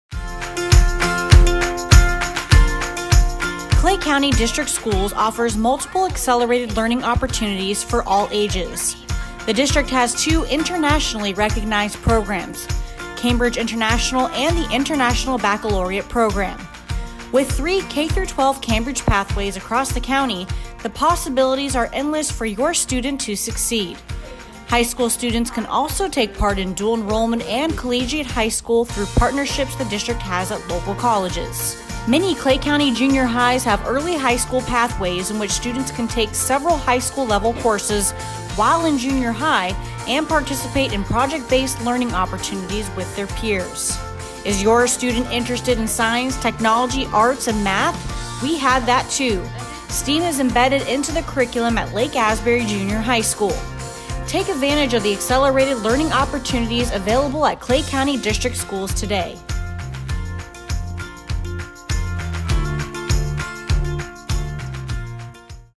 National School Choice Week: Accelerated Learning
National School Choice Week is January 24-29, 2021! We will be showcasing different choice programs we offer across the district. This video is all about our Accelerated Learning Options!

for more information on our offerings.